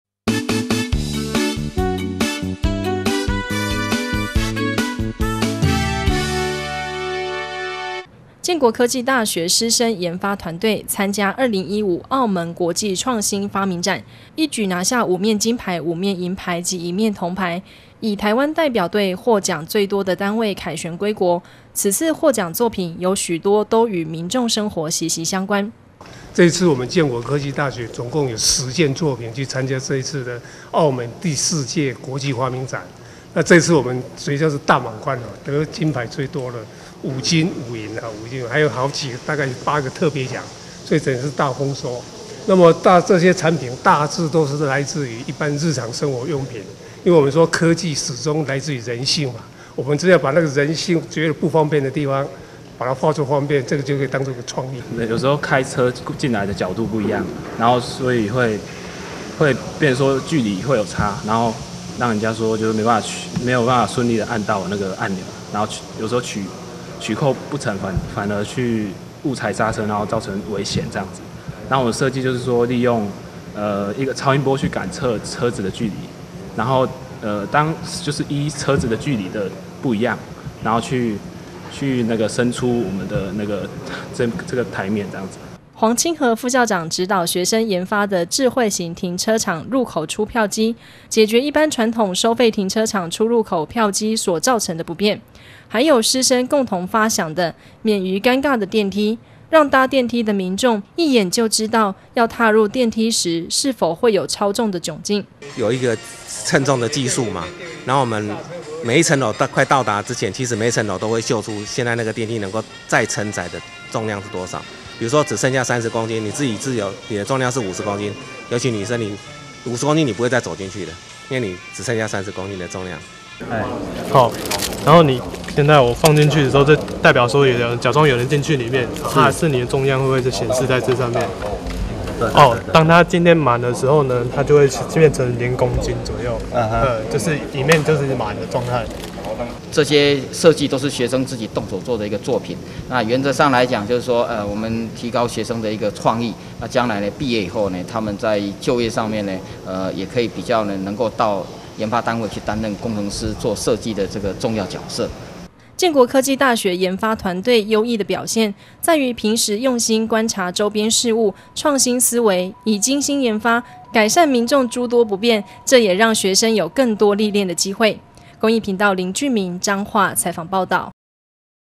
104-07-09 澳門國際發明賽 建國科大奪5金5銀
建國科技大學師生研發團隊，參加2015澳門國際創新發明展，一舉拿下五面金牌五面銀牌及一面銅牌，以台灣代表隊獲獎最多的單位凱旋歸國，此次獲獎作品有許多都與民眾生活息息相關。

  黃清和副校長指導學生研發的智慧型停車場入口出票機，解決一般傳統收費停車場出入口票機所造成的不便；還有師生共同發想的「免於尷尬的電梯」，讓搭電梯的民眾，一眼就知道，要踏入電梯時是否會有超重的窘境。
  建國科技大學研發團隊優異的表現，在於平時用心觀察週邊事物，創新思維、以精心研發，改善民眾諸多不便，這也讓學生有更多歷練的機會。